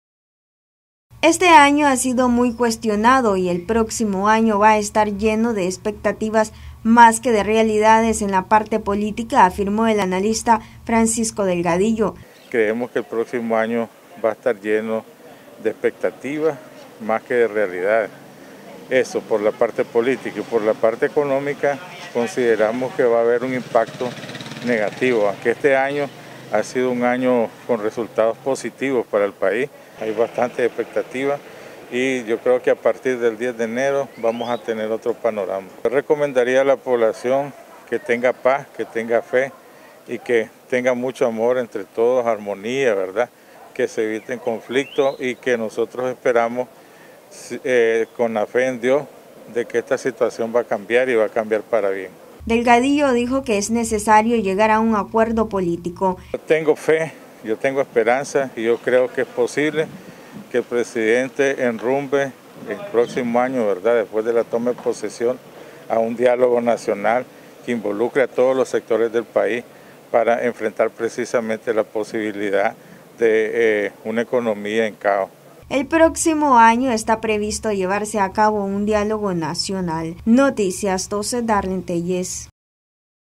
El país con grandes expectativas políticas para el próximo 2022
Noticias12Nicaragua Analista político Francisco Delgadillo, afirma que hay expectativa para el 2022 tanto en el mundo político como económico.